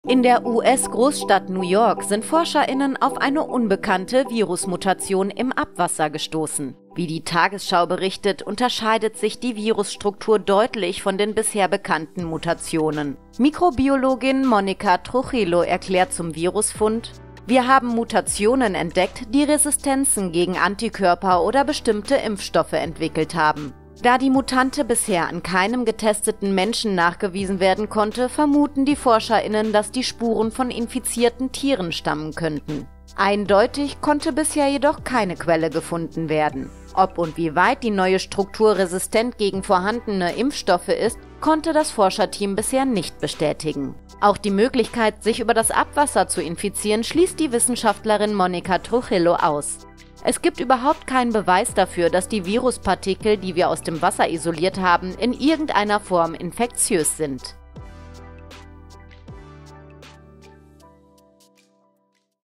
Corona-Mutante aus New York: Wie gefährlich ist die neue Variante?
In der Stadt New York sind Forscher:innen jetzt auf eine unbekannte Virus-Mutation im Abwasser gestoßen. Ob und inwieweit die neue Struktur gegen Impfstoffe und Antikörper resistent ist, wird jetzt geprüft.